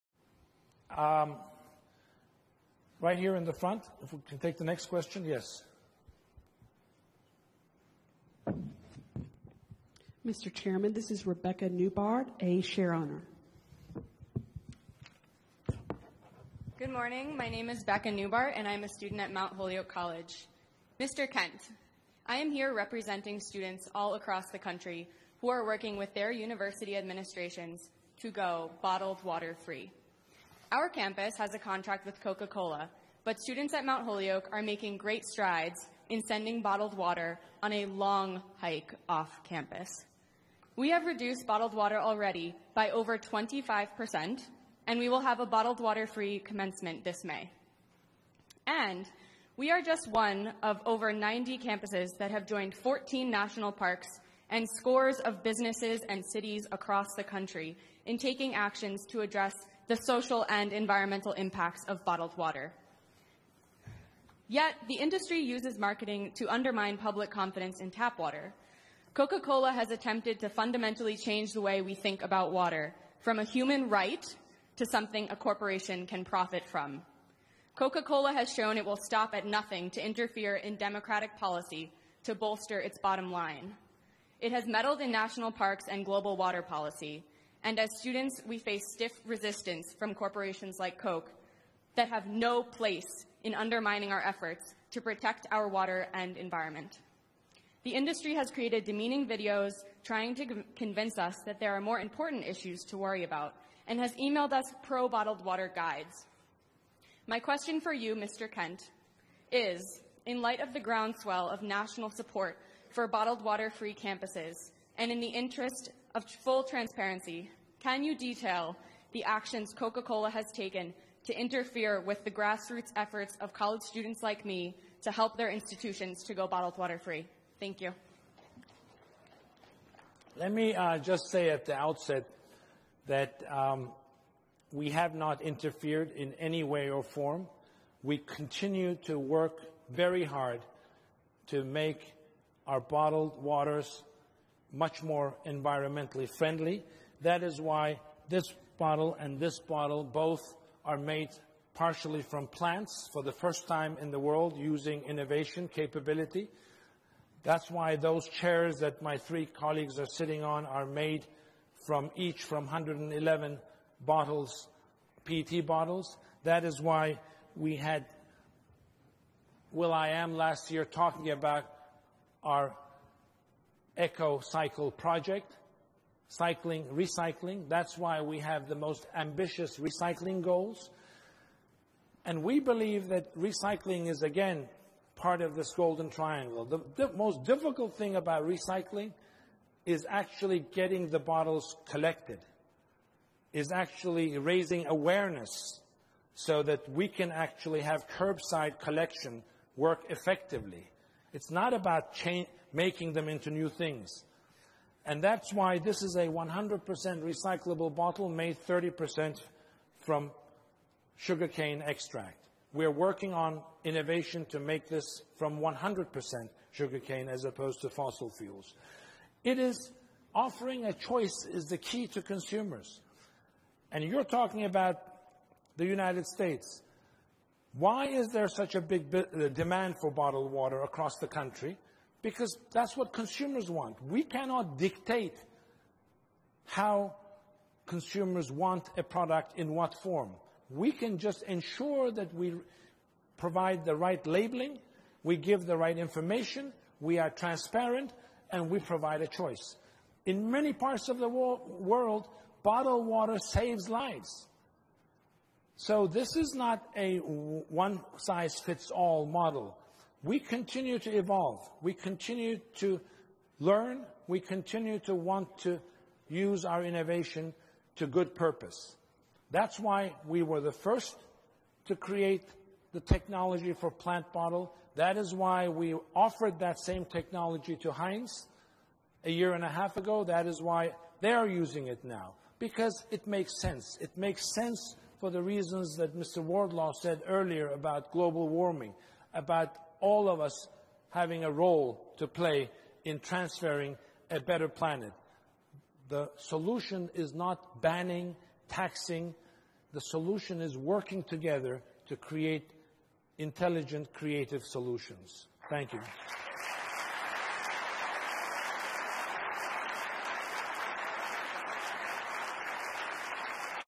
Rebecca Neubar Questioning Coke CEO Muhtar Kent About Bottled H2O
Rebecca Neubardt, Mt. Holyoke College Student and Corporate Accountability Activist Challenging CEO Muhtar Kent on the bottled water issue.